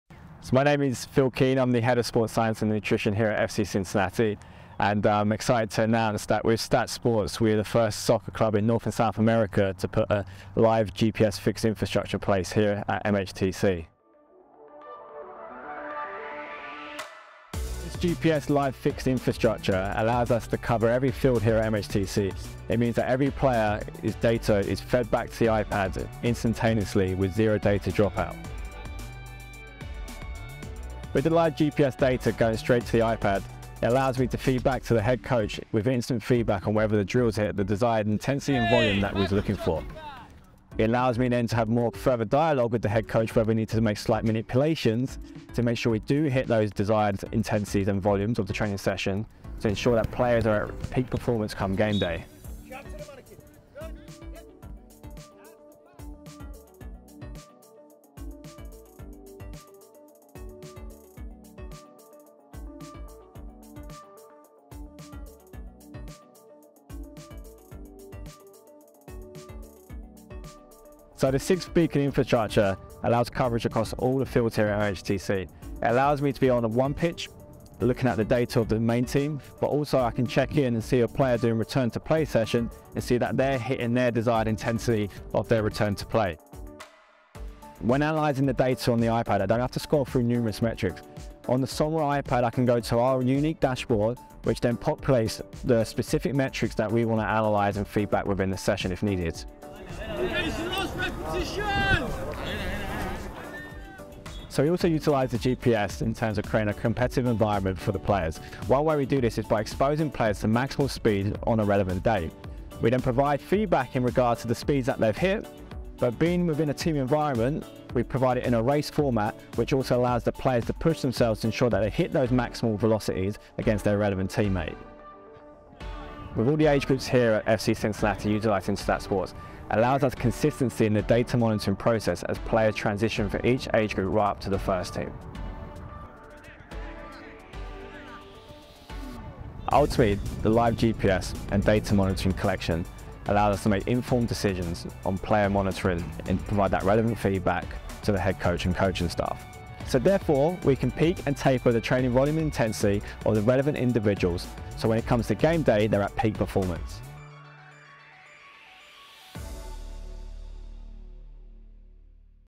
FC Cincinnati x STATSports
FC Cincinnati's Phil Keehne (Head of Sports Science and Nutrition) gives a run-down on why they are the first Soccer Club in North & South America to install STATSports Fixed Infrastructure.